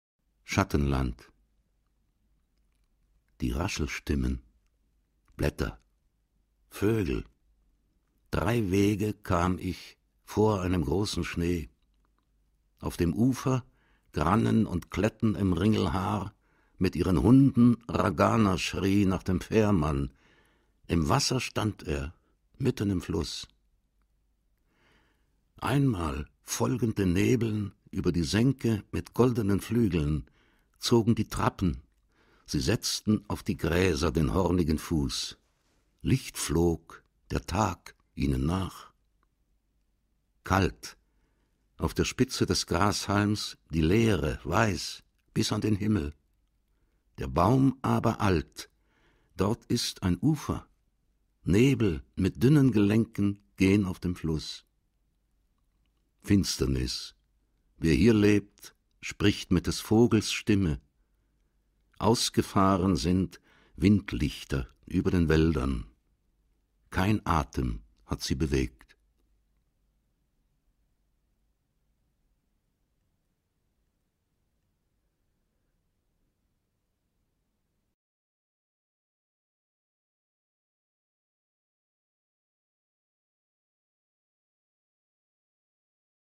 Johannes Bobrowski „Schattenland"
Rezitation: Jürgen Hentsch
aus dem Hörwerk
„Lauter Lyrik – der Hör-Conrady“
erschienen im Patmos Verlag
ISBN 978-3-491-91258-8

Text:

Die Raschelstimmen,
Blätter, Vögel, drei Wege
kam ich
vor einem großen Schnee.
Auf dem Ufer, Grannen und Kletten
im Ringelhaar, mit ihren Hunden
Ragana schrie nach dem Fährmann, im Wasser
stand er, mitten im Fluß.

Einmal,
folgend den Nebeln,
über die Senke mit goldenen Flügeln
zogen die Trappen, sie setzten
auf die Gräser den hornigen Fuß,
Licht flog, der Tag ihnen nach.

Kalt. Auf der Spitze des Grashalms
die Leere weiß
bis an den Himmel. Der Baum
aber alt, dort ist
ein Ufer, Nebel mit dünnen
Gelenken gehn auf dem Fluß.

Finsternis, wer hier lebt,
spricht mit des Vogels Stimme.
Ausgefahren sind
Windlichter über den Wäldern.
Kein Atem hat sie bewegt.

Bilder: Fotocollage